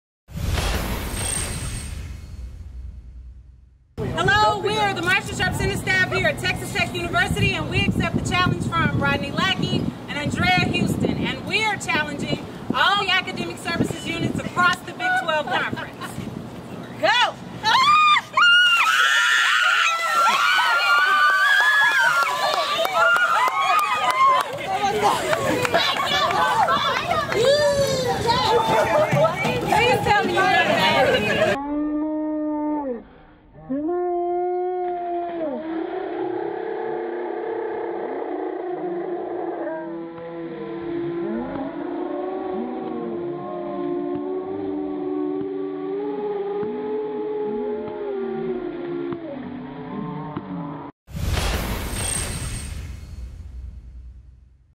Marsha Sharp Center Staff at Texas Tech Takes
The Marsha Sharp Center for Student Athletes staff accepts the ALSIceBucketChallenge and issues their own to all athletics academic staffs in the Big 12 Conference.